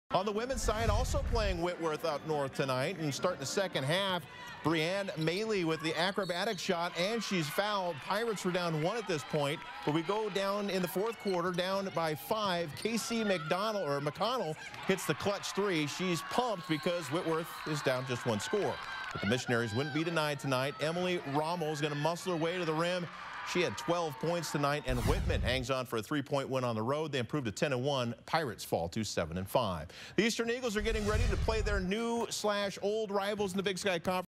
McConnell, Whitworth come up short against Whitman
KC McConnells desperation three-pointer as time expired was no good,  allowing visiting Whitman to escape with a 55-52 Northwest Conference  womens basketball victory over Whitworth on Tuesday night in the  Whitworth Fieldhouse.